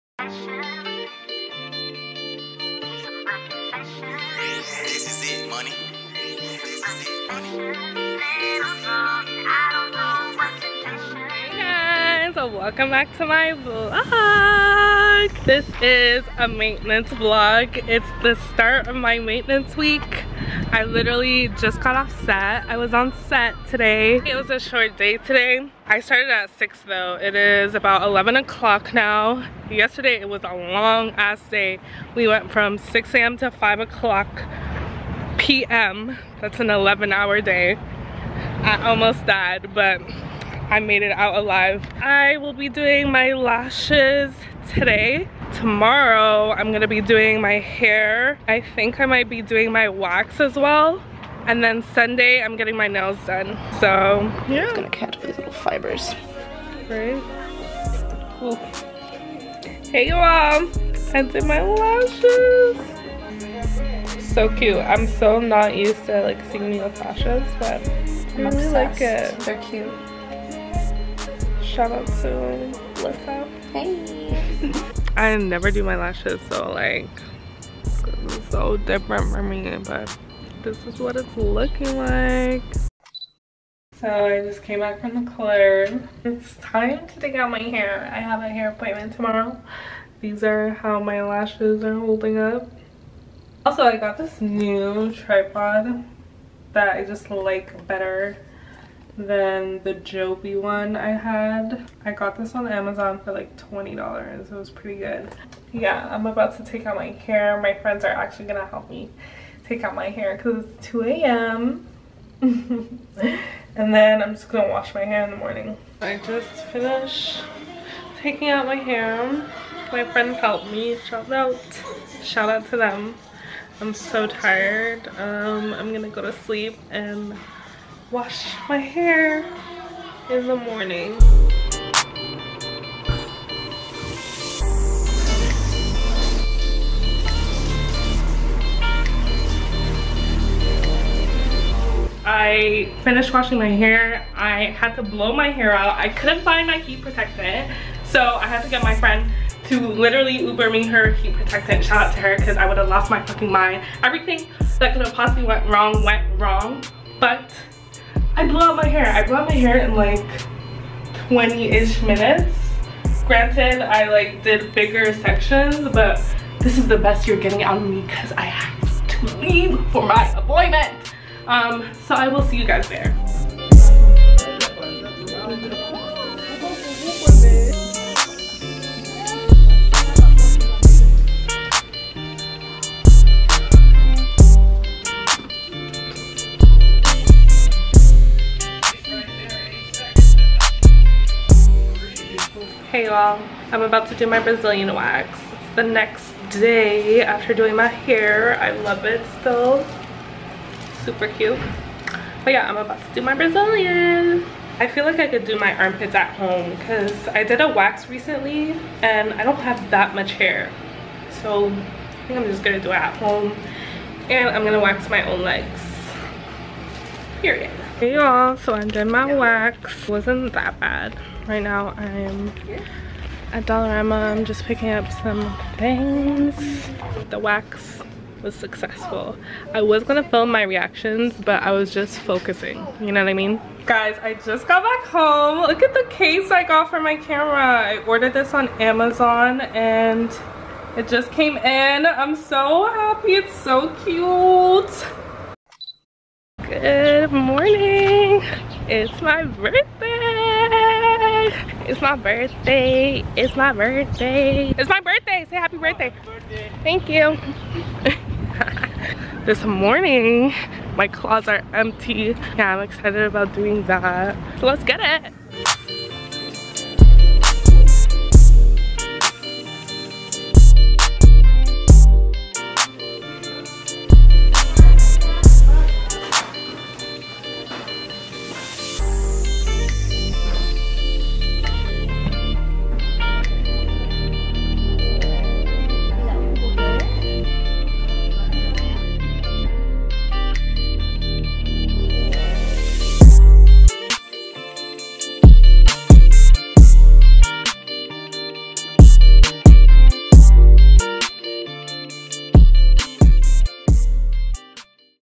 THE BIG 3 | MAINTENANCE VLOG | hair, lashes, nails + wax
Join me on my maintenance week before my birthday 🤍
I was super happy with all of the services I received, if you missed it in the video, here’s where to find them on Instagram:

Lashes @lashpluglyss
Nails @pedinnails_dundaseast
Hair @sussyhairbraiding